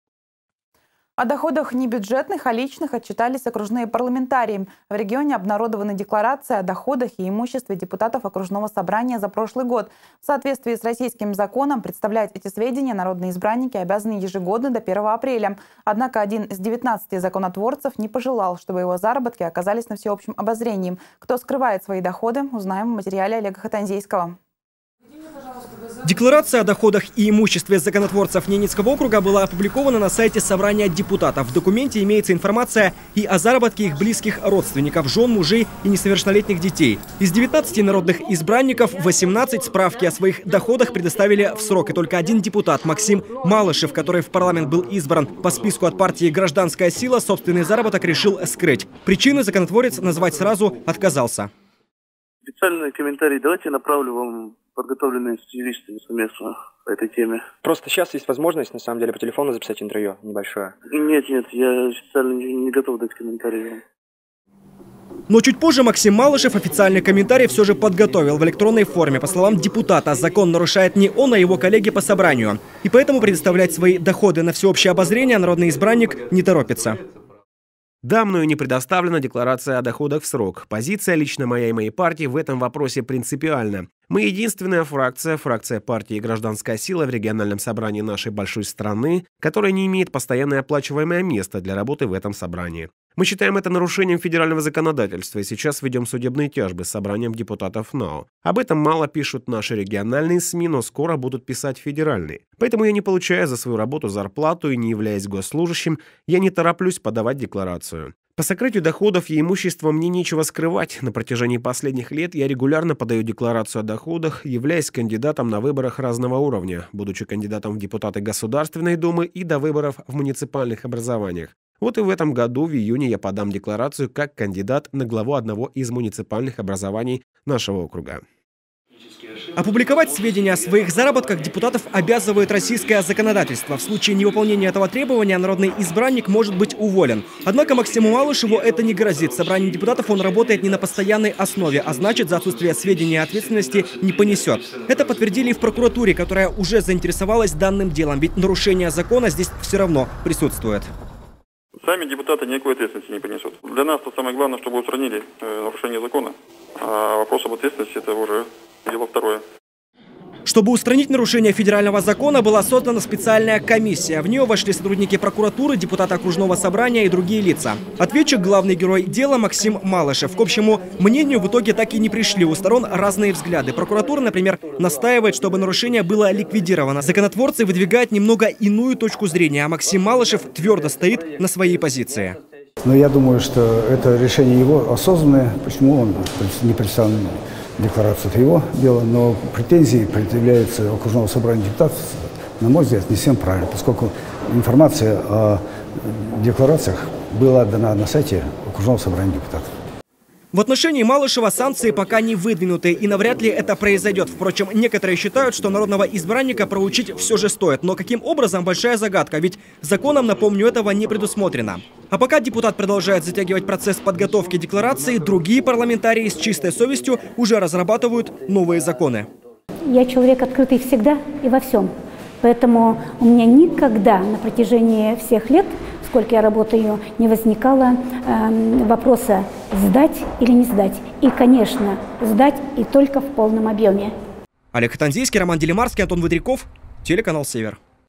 Не все депутаты НАО отчитались о доходах
О доходах не бюджетных, а личных отчитались региональные парламентарии. В Ненецком округе обнародованы декларации о доходах и имуществе депутатов окружного собрания за прошлый год. В соответствии с российским законом представлять эти сведения народные избранники обязаны ежегодно до 1 апреля. Однако один из 19 законотворцев не пожелал, чтобы его заработки оказались на всеобщем обозрении.